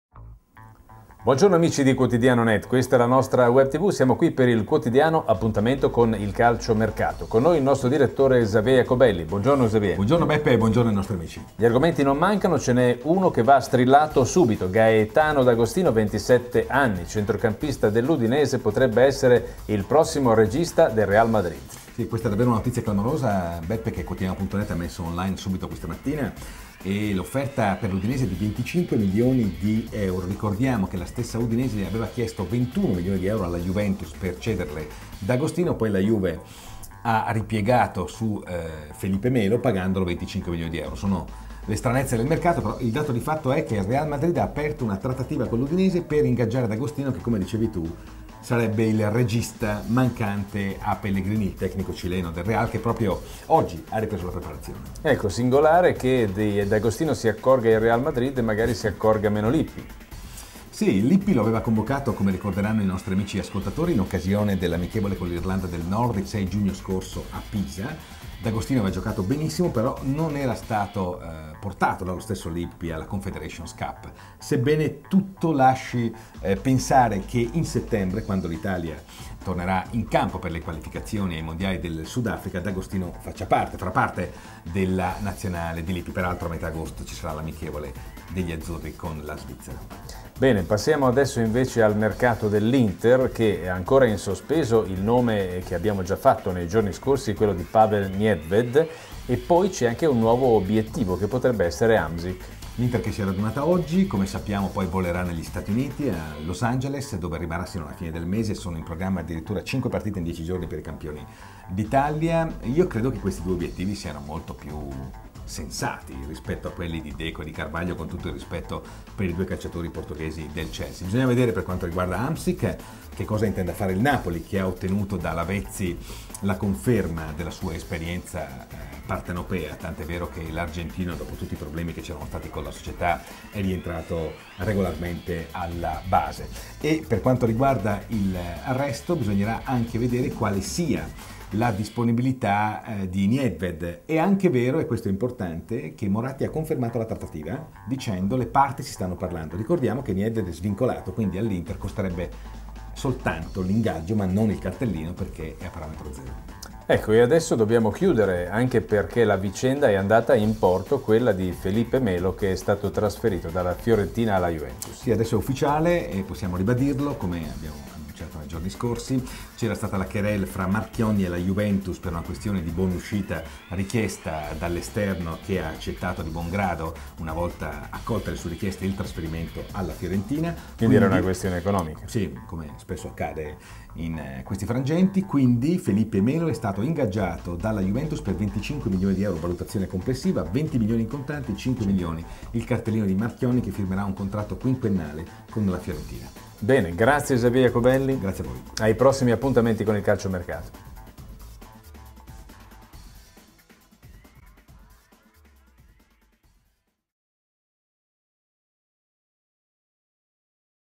Il Real piomba su D'Agostino - L'Inter vuole Hamsik
Commento di Xavier Jacobelli e Giuseppe Tassi